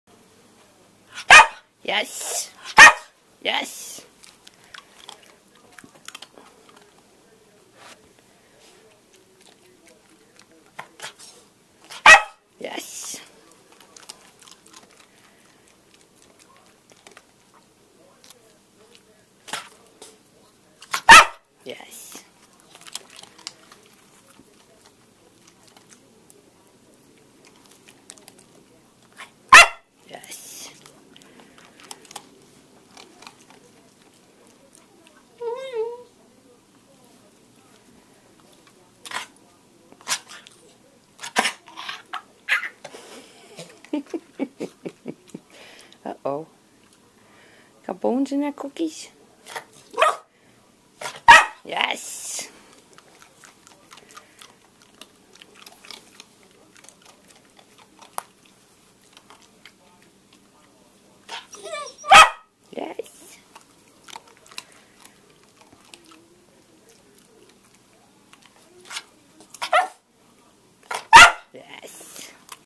PippaLearnsBarking.mp4
Pippa Learns Barking on hand cue - March 29th 2012
She recently found her voice, so I'm "working" with it.